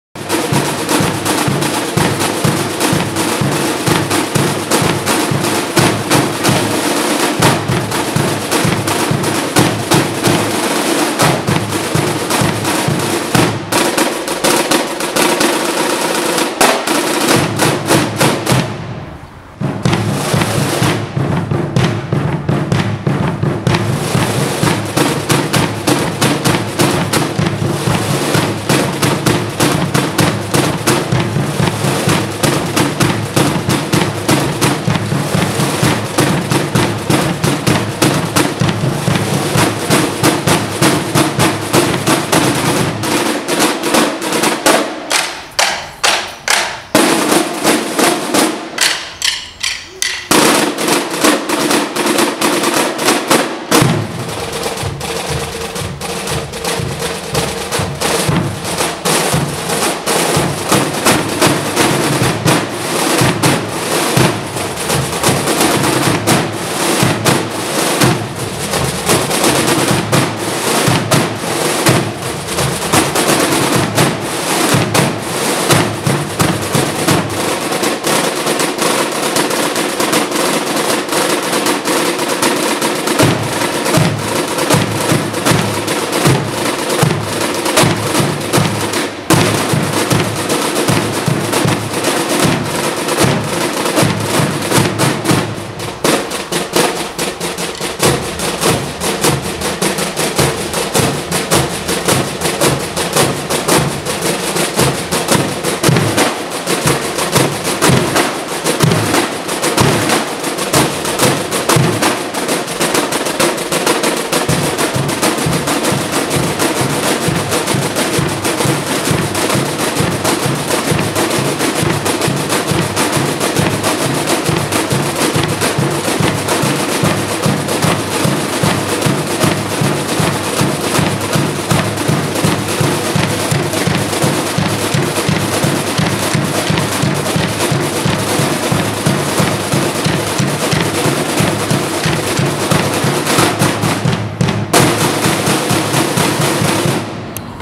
The Royal British Legion Band and Corps of Drums Romford
The Royal British Legion Band and Corps of Drums Romford played a niece piece at The Royal Garrison Church of St. George.
In memory of the 4th Anniversary of Drummer Fusilier Lee Rigby